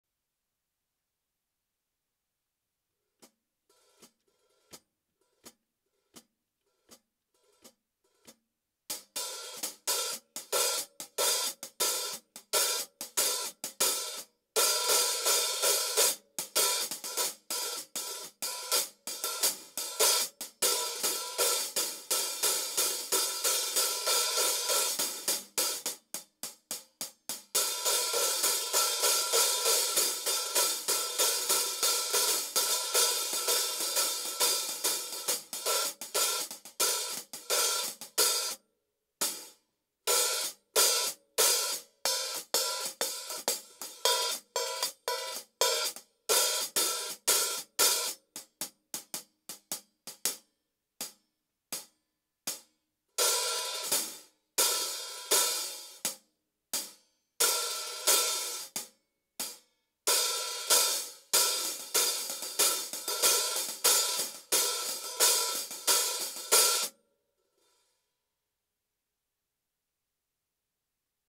14" Sabian HHX Evolution Hi-Hat
In this series of video's I show you my Sabian Cymbals and let you hear what they sound like.

Recorded with just the two EM81 overhead mics i've got!
These mics are included in the T.BONE DC4000 drum mic set.